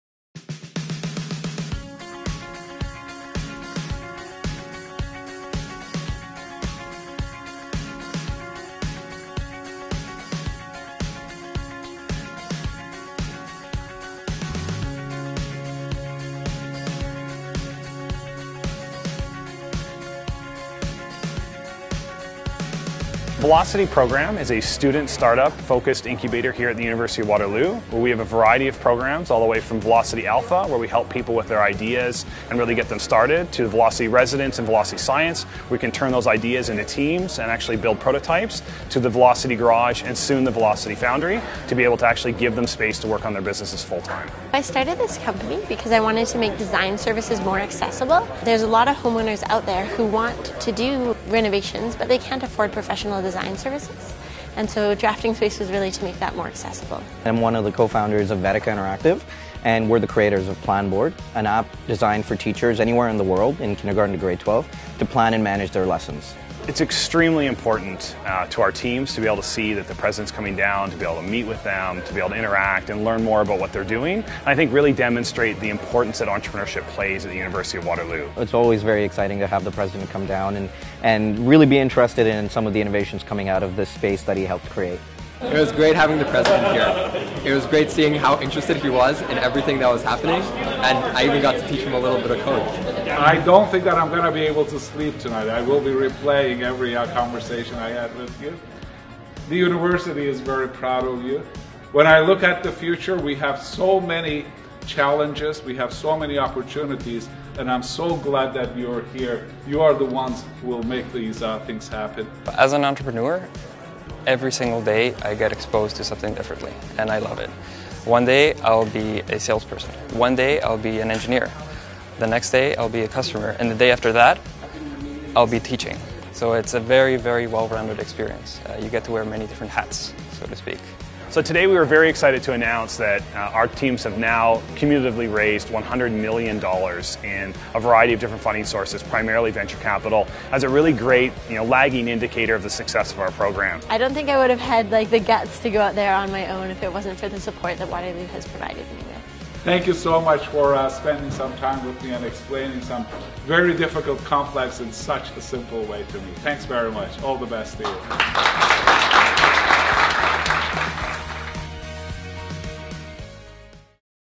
Celebrating Entrepreneurship at Waterloo
Join President Feridun Hamdullahpur on a tour of the University of Waterloo's Velocity Garage, a key component of Waterloo's world-renowned entrepreneurial ecosystem, and share in the news of Velocity's recent funding successes.